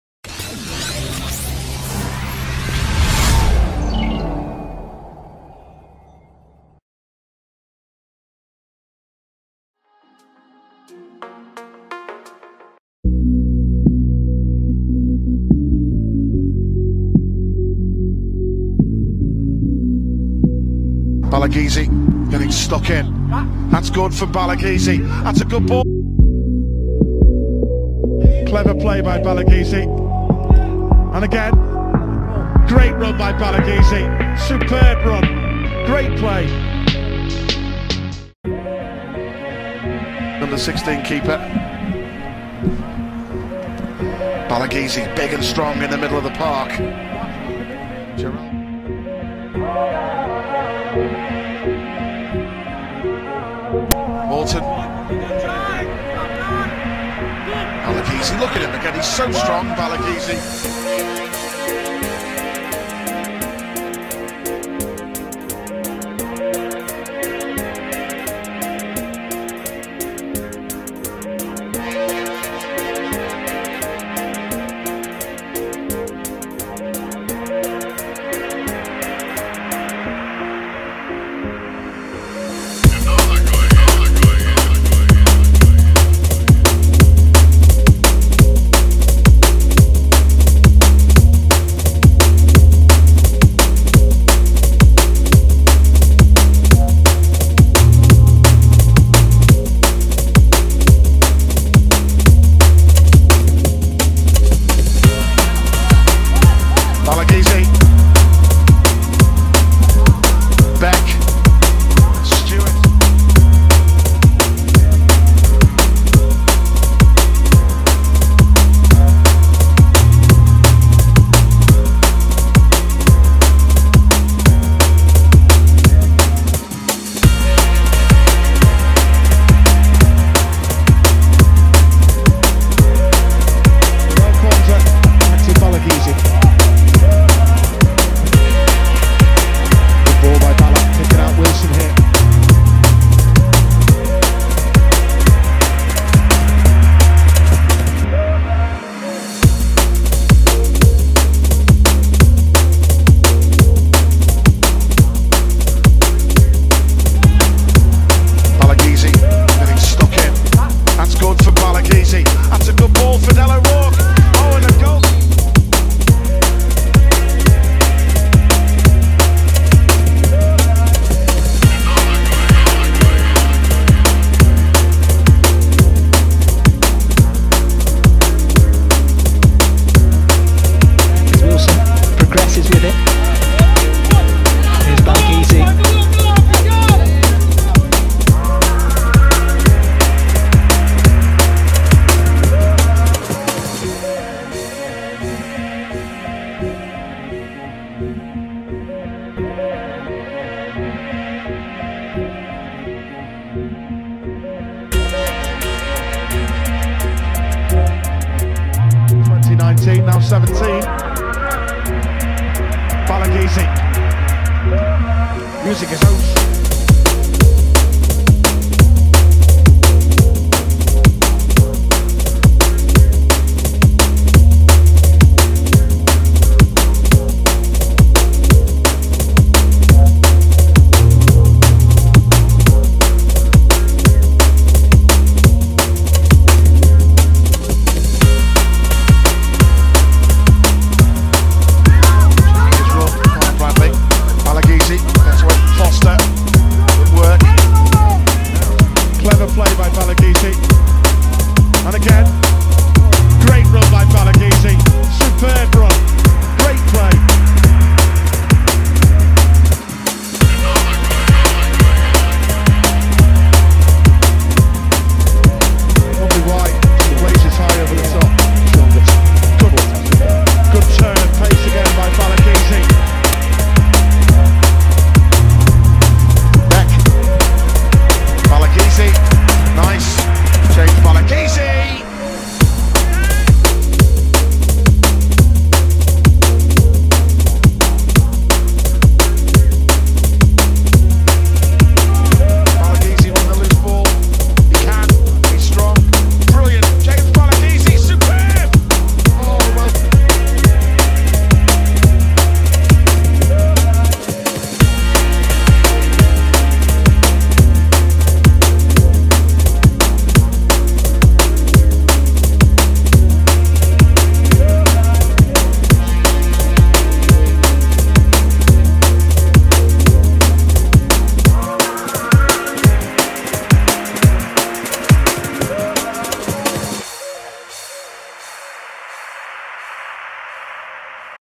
JAMES BALAGIZI ✭ LIVERPOOL ✭ THE NEW ENGLISH WONDERKID ✭ Skills & Goals ✭ 2020 ✭
James Balagizi signed his first professional contract with Liverpool FC in September 2020. An England U17 international who is usually positioned as an attacking midfielder, Balagizi joined the Reds from Manchester City as an U11 player.